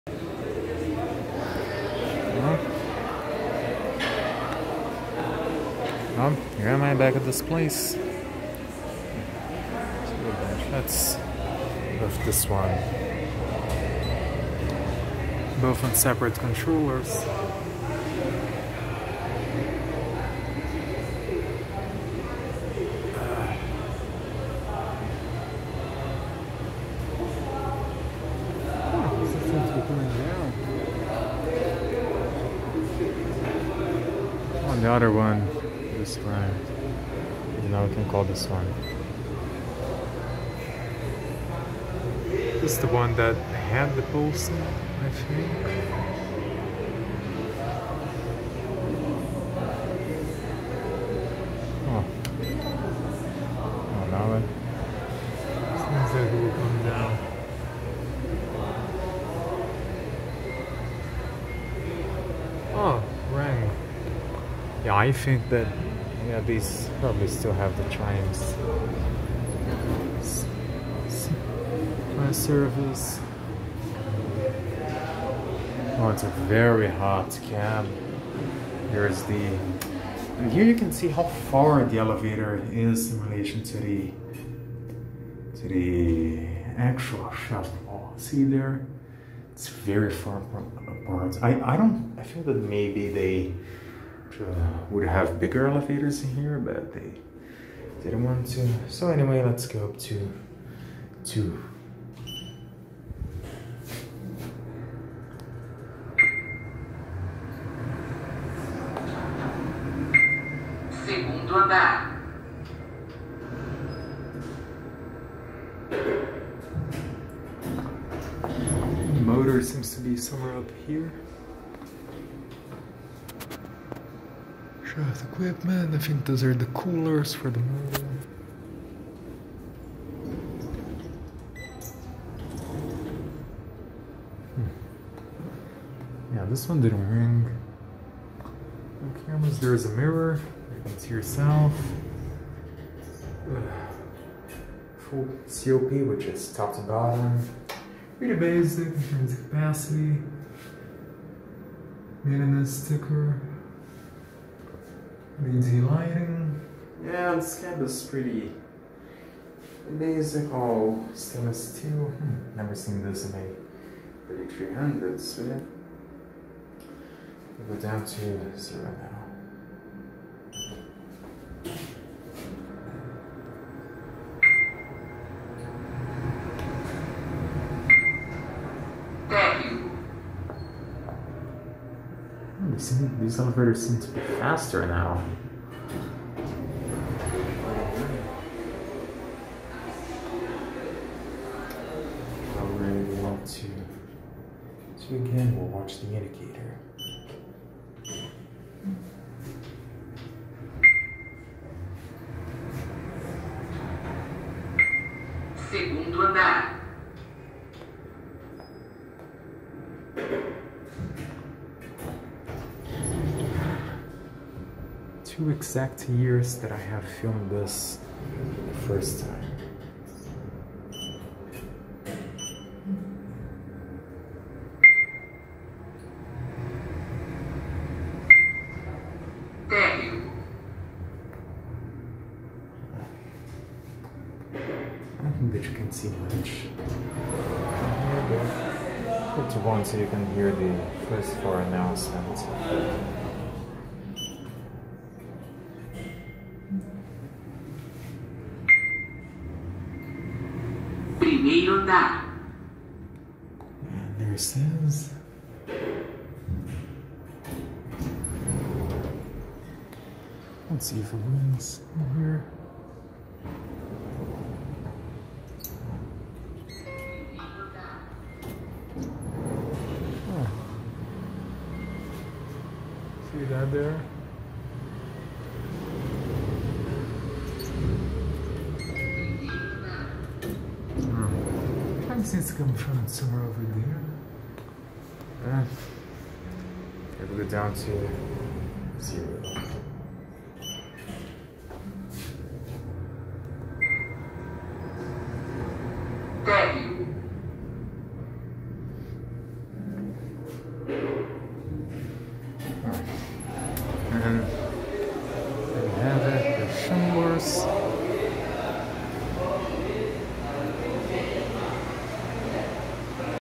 Atlas Schindler 3300 Scenic Elevators - Strip Mall - South Side - MG, BR
A set of two Schindler 3300 elevators in a strip mall in the new prime area of the city. These were some of the first elevators uploaded to my channel, and this time, some of the very last filmed in 2020, just hours before NYE.

Serving as the main passenger elevators, going up to the floors and the bar/restaurant on level 2, which also exits into the upper street that goes behind.
The shaft walls are much bigger, which indicates that it was probably intended to have bigger cabs, but decided for smaller ones. A good decision, as they don't handle much traffic.
The cabs are pretty plain, in good shape for exterior elevators and in an application like this.
These FI GS totem-type COPs aren't all that common here. The voice and floor passing chime are also present, and like many that I find, very loud. One other thing that's also becoming popular with Schindler recently over here, are the american-style braille plates, which are placed in the jamb near the call station, instead of the proper sides. It's actually meant to show which floor you're in from the outside, as many people don't pay attention to that.
Overall, pretty basic cabs and install.

There is a third elevator here on the other side, another 3300, just enclosed in the back of a corridor, used more for service despite being public.

Both run very well, with no problems at all, giving off a nice ride as expected. They have that running noise that most of these end up having. The car I filmed makes the loud buzzing sound that some 3300 motors have when going down, I only found that in some other 1 or 2 of this model. The parking brakes can also be heard pretty well on them. As these run in large shafts, their sound ressonates and makes the noise louder.
One issue I have with them, is the controller, they seem to be on the same system, but are a bit confusing as to which one is answering the calls, as the chimes don't always play and they're a bit far from each other. Not that big of a deal, but people who don't use them much might get confused...

Specifications
Manufacturer: Atlas Schindler
Mantainer: Atlas Schindler
Model: 3300
Fixtures: FI GS (totem version)
Type: Passenger Commercial
Hoisting: MRL Traction
Doors: Center-opening
Capacity: 975 kg/13 persons (apx. 2150 lbs.)
In Service: 2016
Floors Served: 2 (0* - 1 - 2)
Operation: Digital controller, VF-driven motor
Rating: 8/10 - Pretty cool elevators, they're nice in a place like this with the glass back. Got some nice motors and run well.

Shot: December 31, 2020
Camera: Samsung Galaxy A71